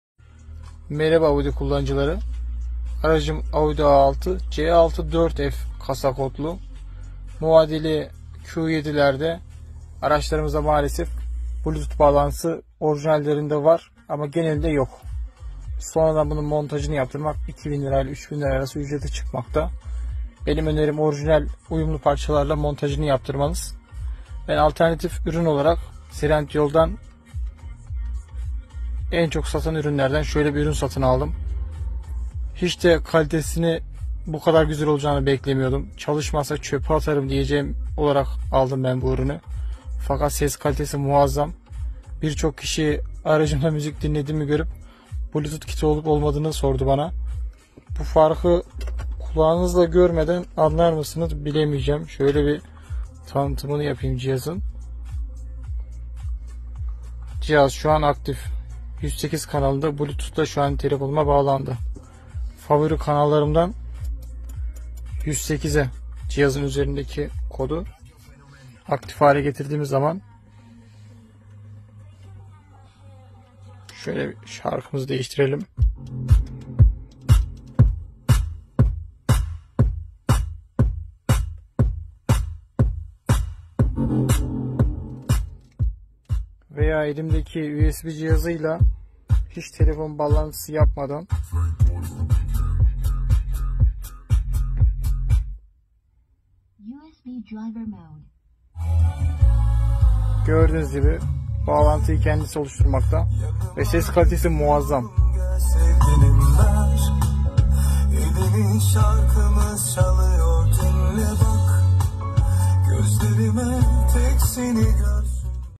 Audi A6 | Her otomobil ile uyumlu Araç Bluetooth bağlantısı (150tl) 🫡🌟
AUDİ A6 C6 4F
3.0TDİ V6 QUATTRO
STAGE 1+ /NO EGR/ NO DPF
300HP 600NM

3.0TDİ 225HP BMK
6 İLERİ ZF TORKKONVERTÖRLÜ TİPTRONİC ŞANZUMAN